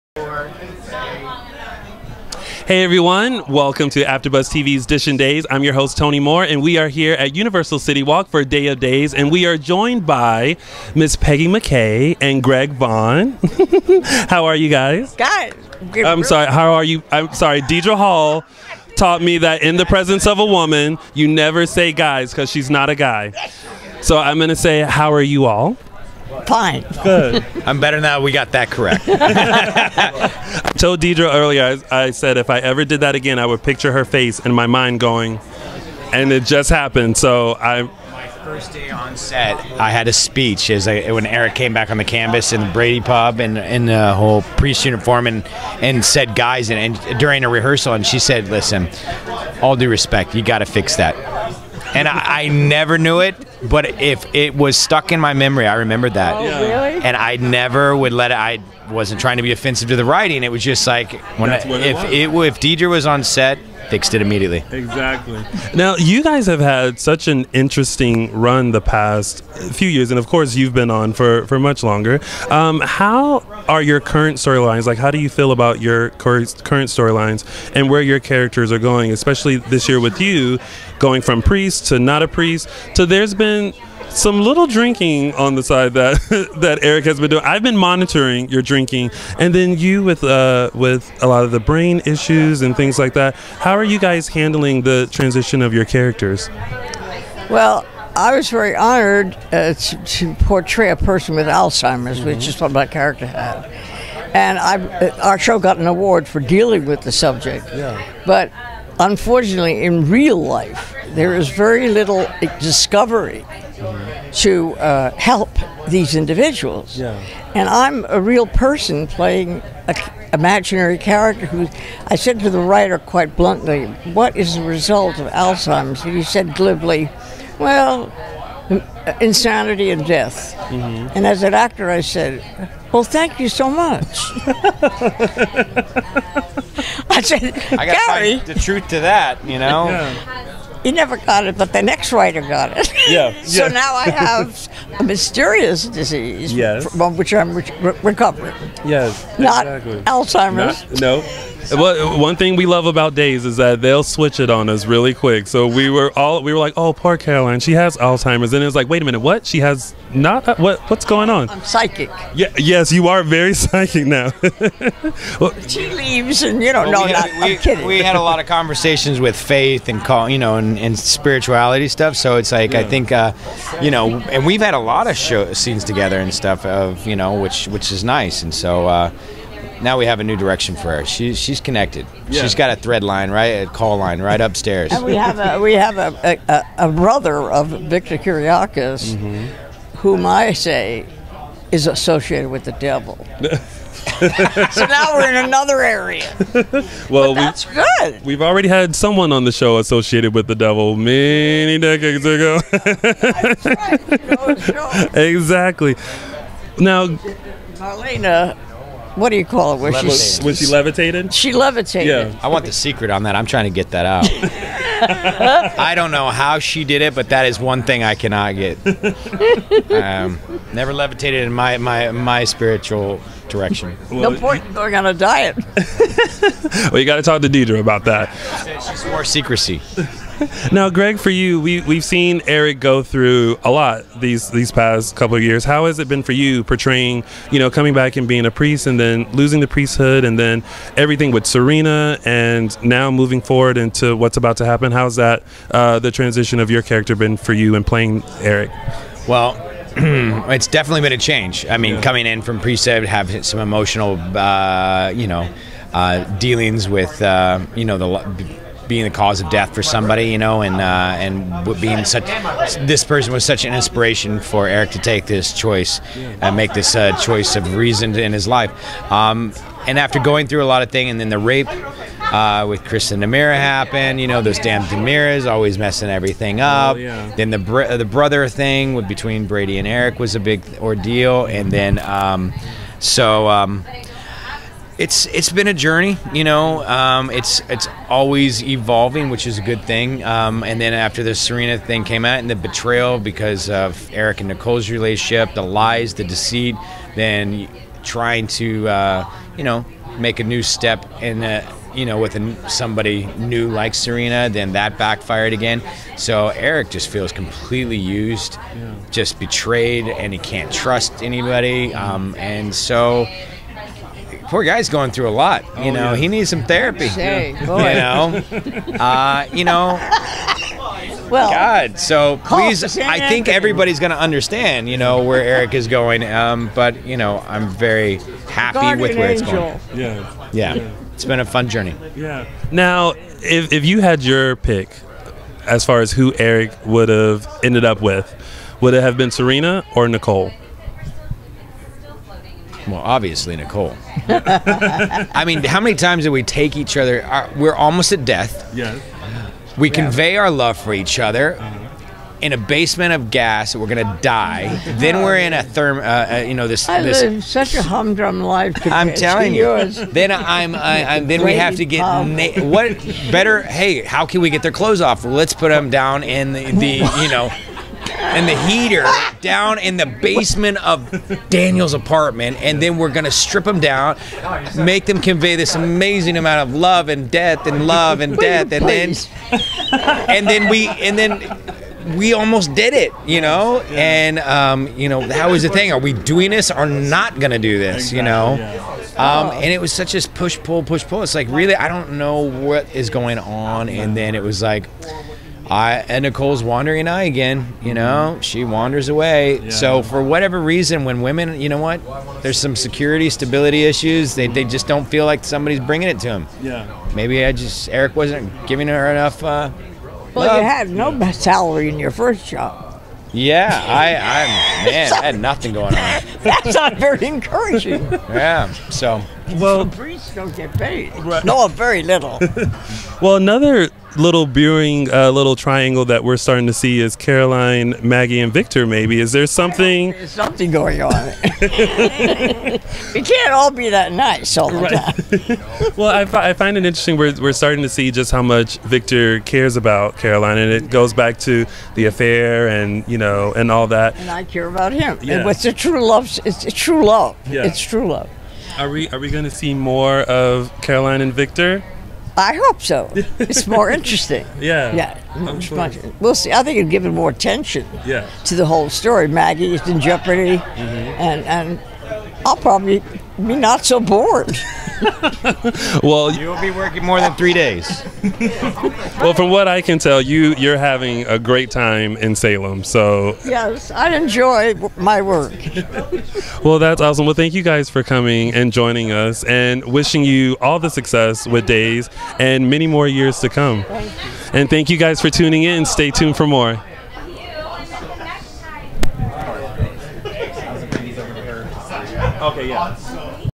2015 Day of Days Event with Peggy McCay and Greg Vaughan at Universal City Walk
Tony Moore from Dishin Days sits down with Peggy McCay (Caroline Brady) and Greg Vaughan (Eric Brady)  from Days of Our Lives  at the 2015 Day of Days Event at Universal City Walk